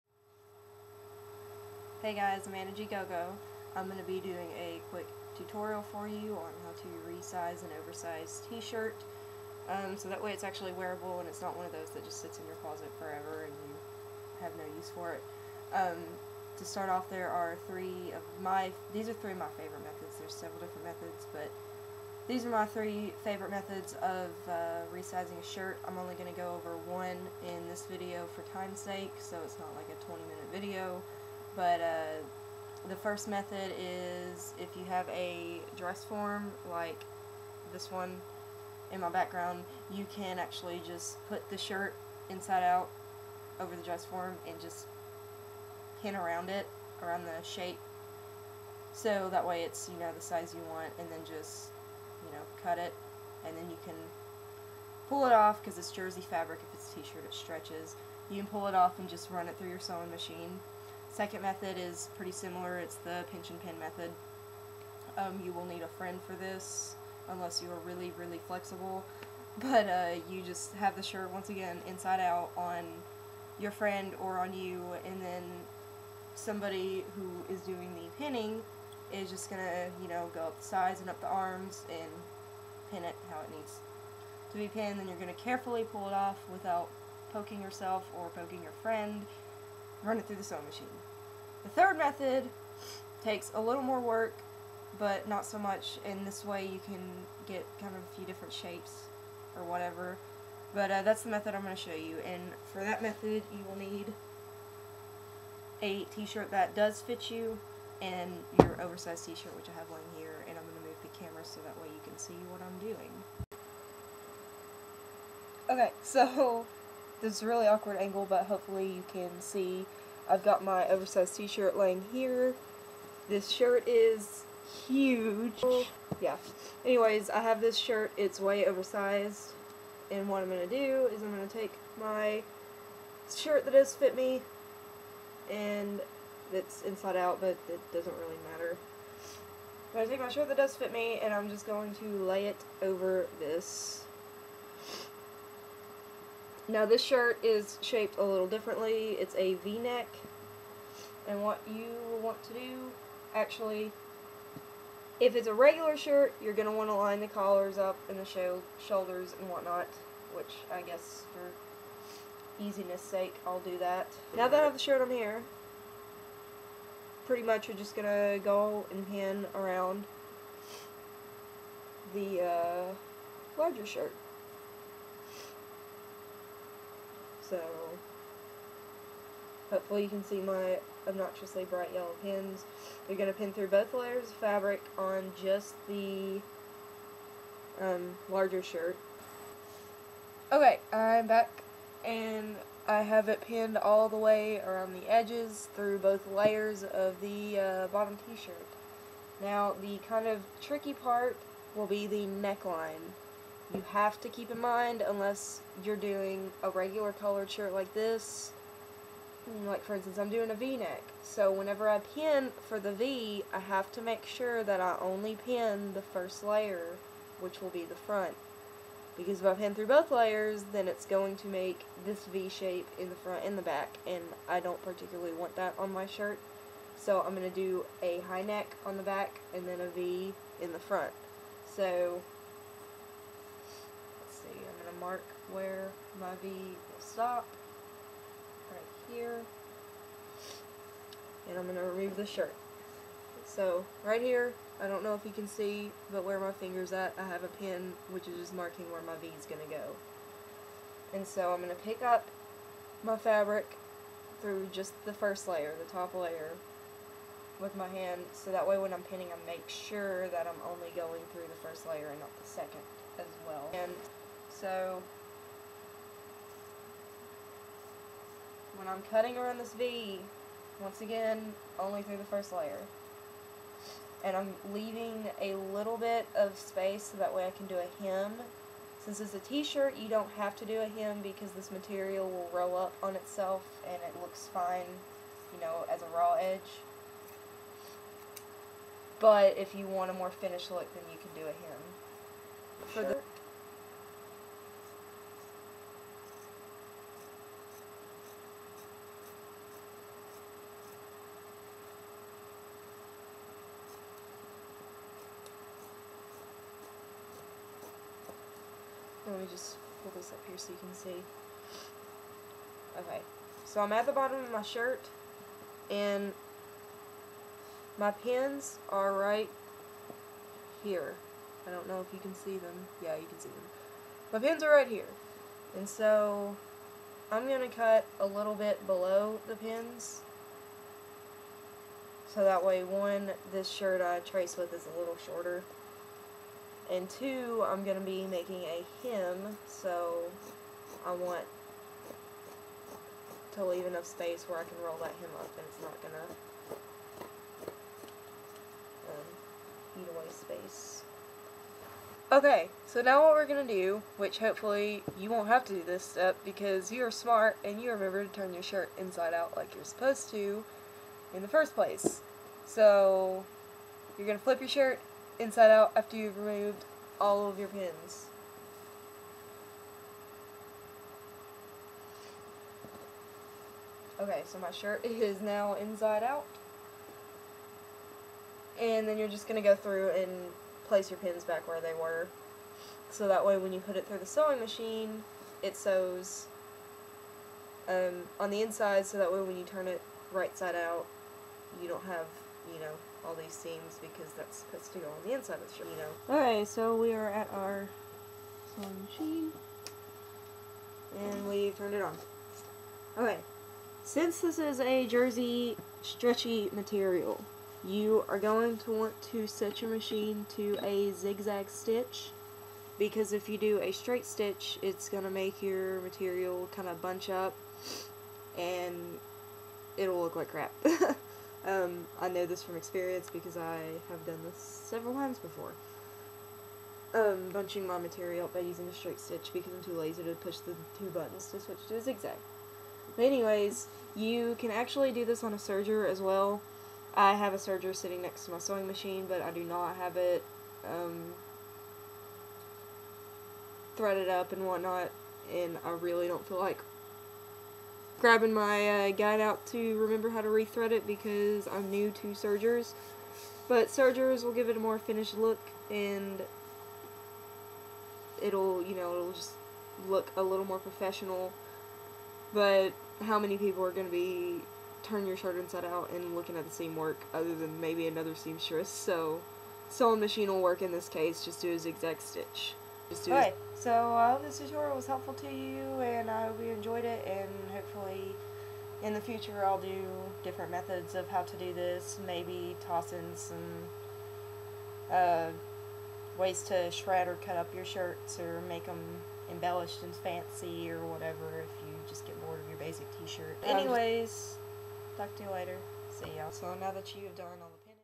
how to: Resize a too big shirt!
Whooo! Yep, a tutorial on a way to resize your shirt.
A few different methods are given.